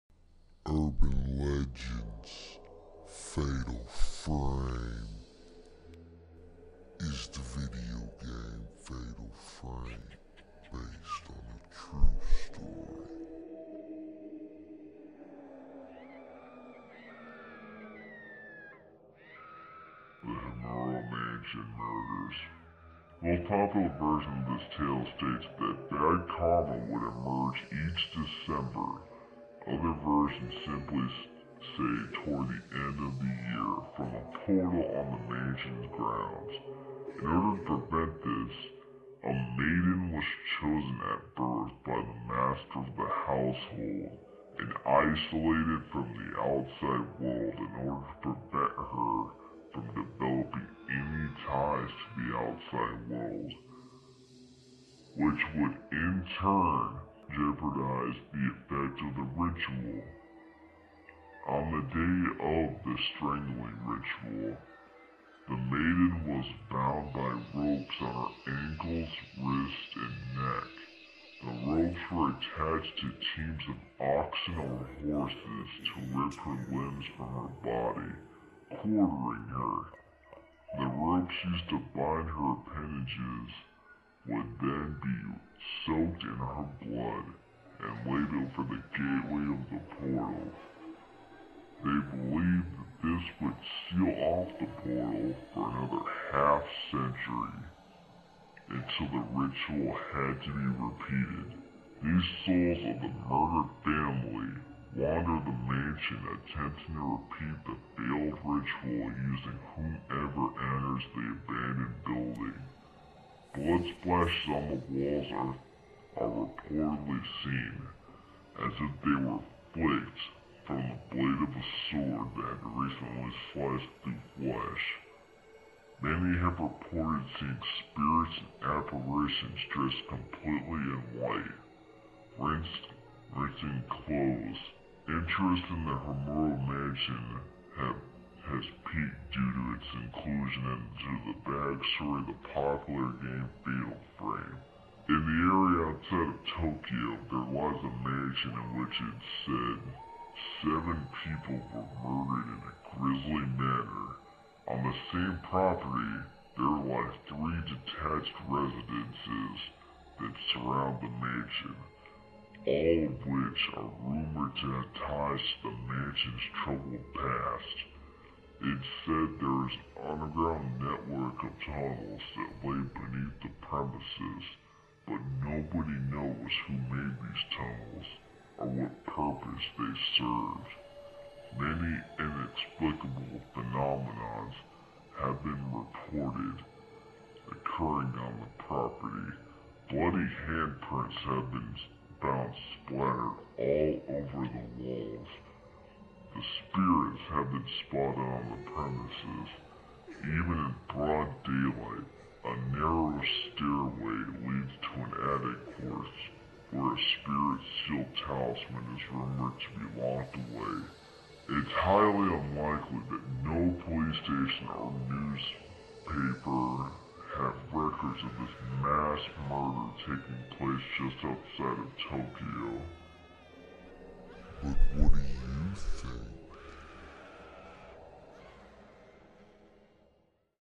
Urban Legend: Fatal Frame (Video Game Edition)
Is the video game 'Fatal Frame' really based on a true story? Parts of it are said to have ties to the 'Himuro Mansion Murders' and other lore.

2013 R.i.P Records Entertainment.